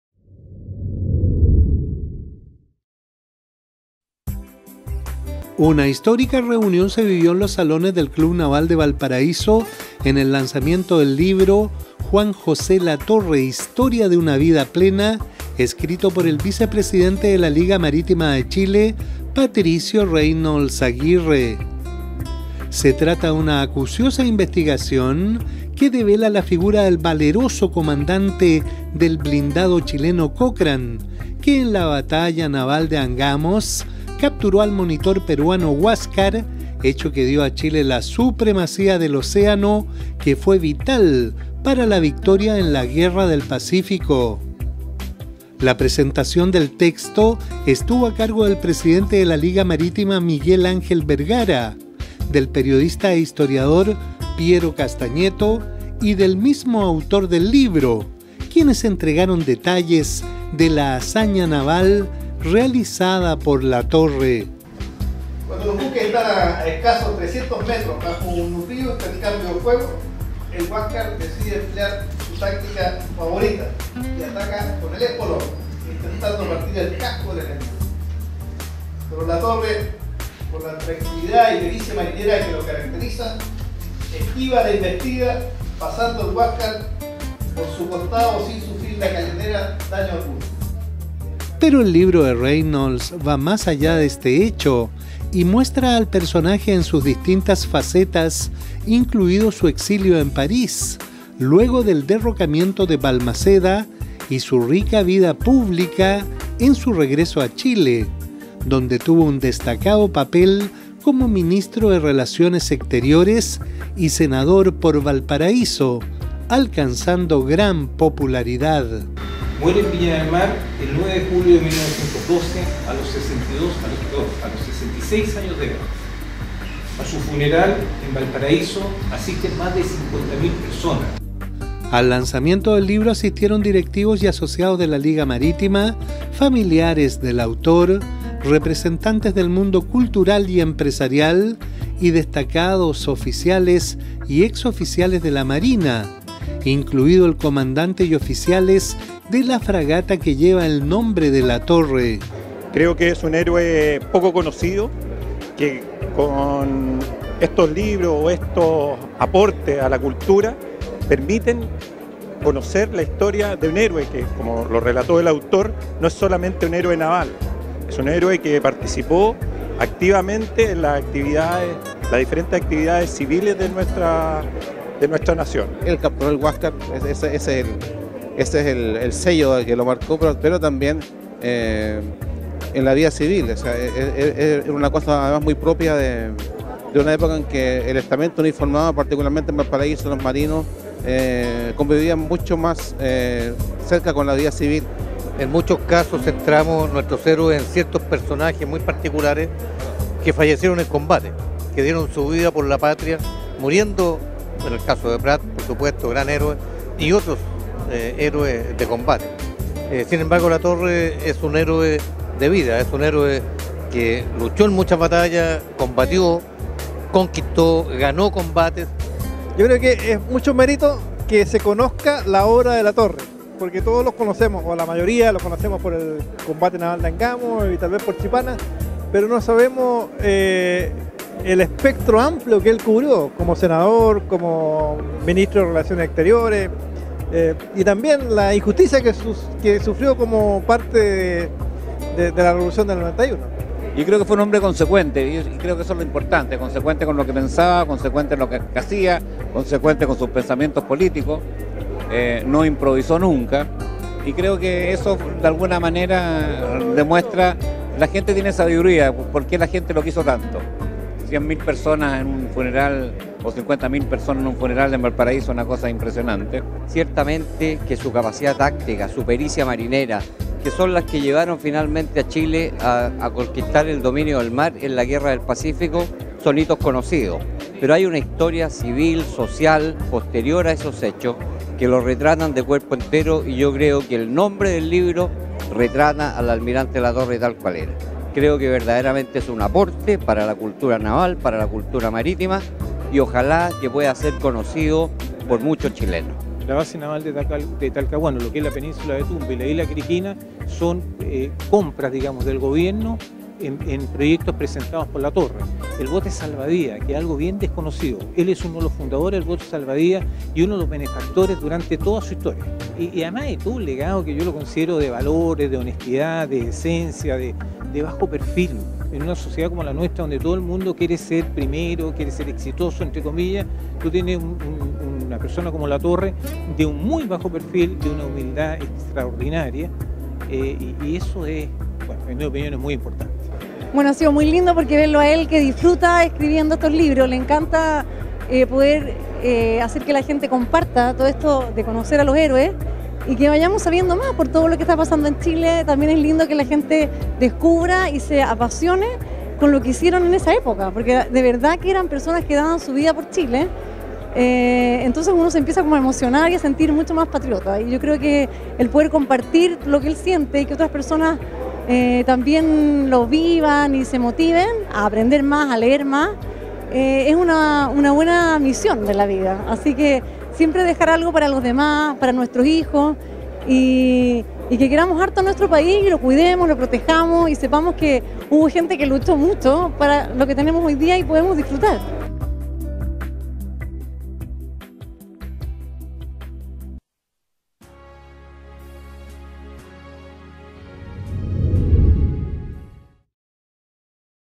La desconocida dimensión del héroe chileno Juan José Latorre
La desconocida dimensión del héroe naval que capturó el monitor Huáscar da a conocer en libro "Juan José Latorre Historia de Una Vida Plena"